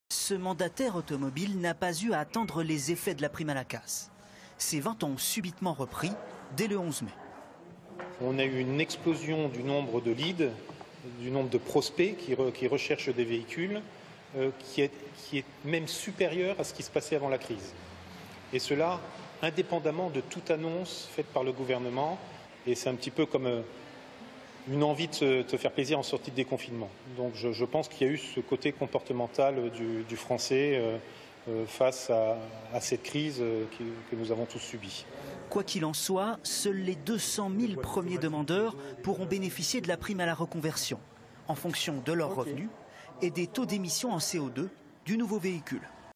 France 3 Franche-Comté 2 juin 2020 - prime à la conversion et reprise après confinement
L'annonce des nouvelles réglementations concernant la prime à la conversion et le bonus écologique a permis une relance considérable des ventes automobiles.
Chez AutoJM, les ventes de voitures ont repris dès la fin du confinement et avant même la réforme de la prime à la casse.
Le nombre de Leads, c'est-à-dire de devis envoyés depuis le site internet autojm.fr a explosé en période de confinement, et a atteint des records dès le confinement levé.
Les professionnels de la vente automobile arriveront-ils à relever la pente pour finir au mieux cette année 2020 ?